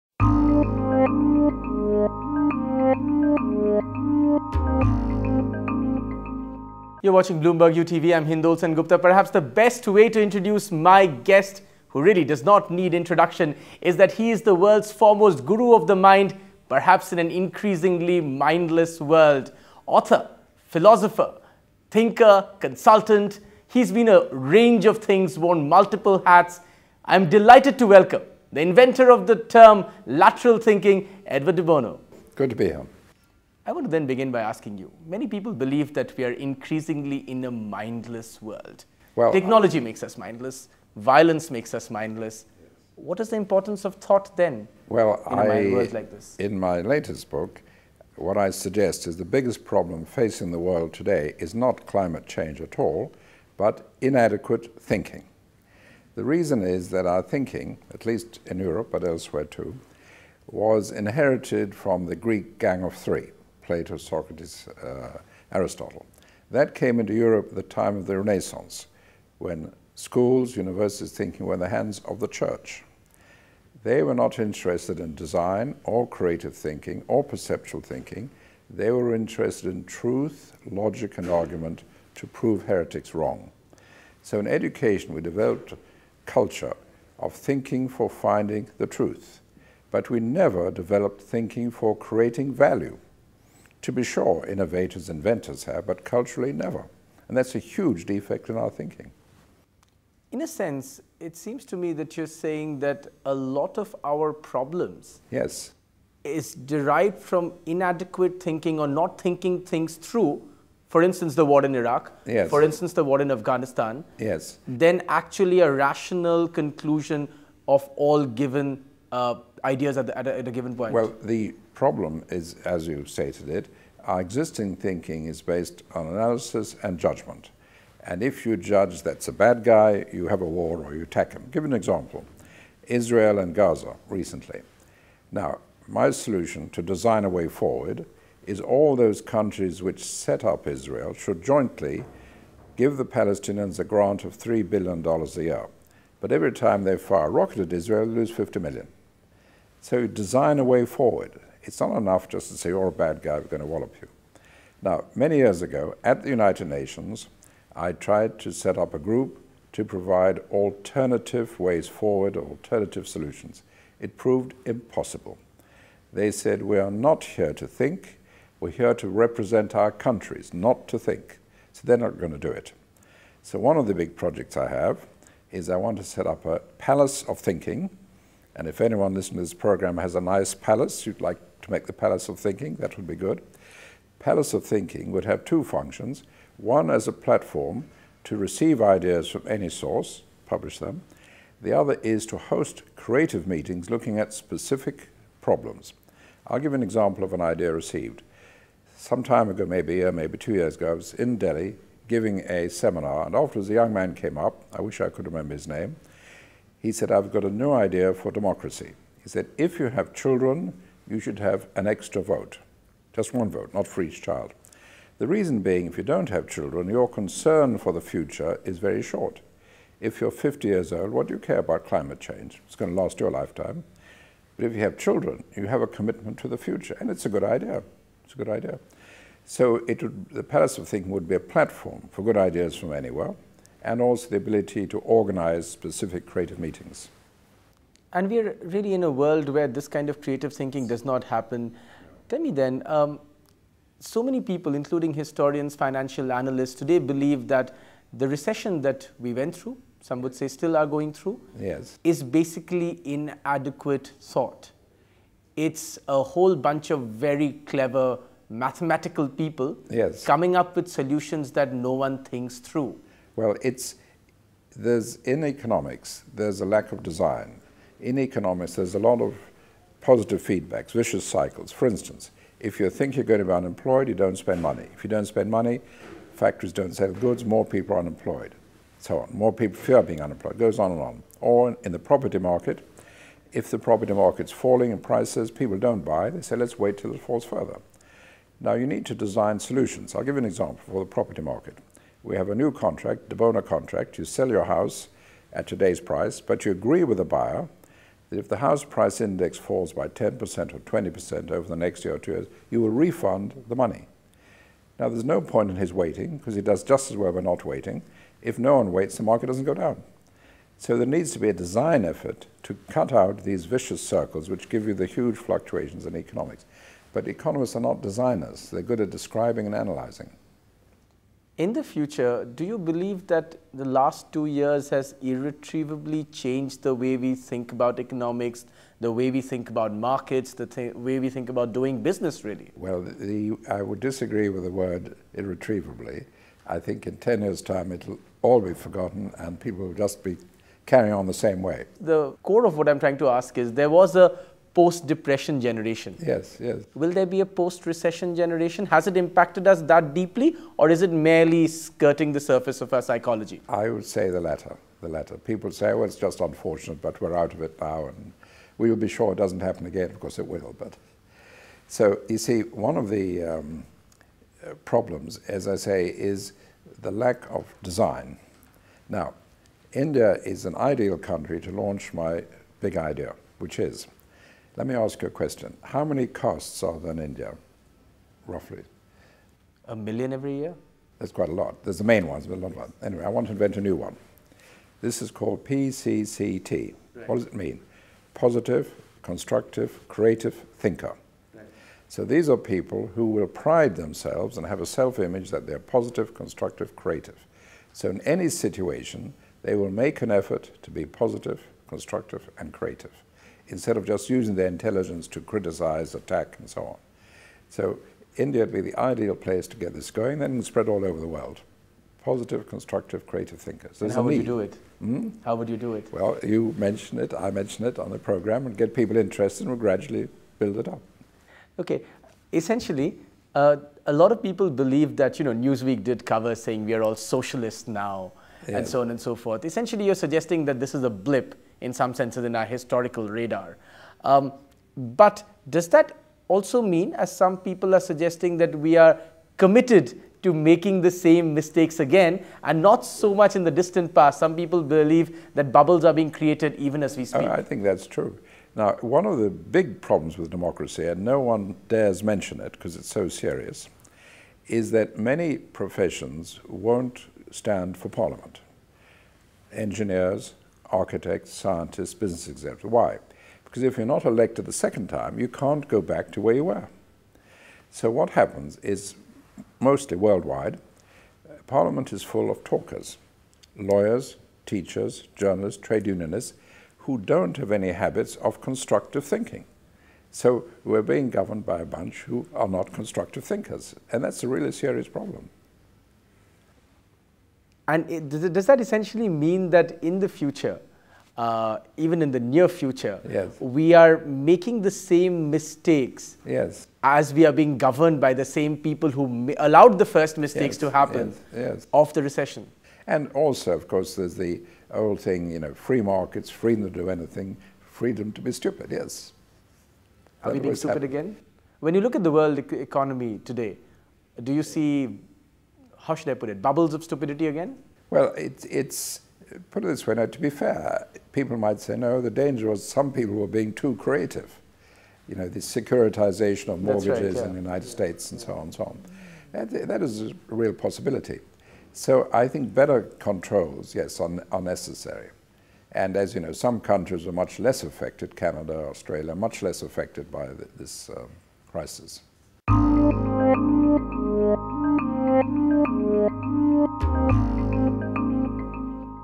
Lateral Thinking with Edward de Bono
One of the world's most influential thinkers Edward de Bono and the father of 'lateral thinking' tells Bloomberg UTV's Hindol Sengupta why the recession was a 'thought problem'!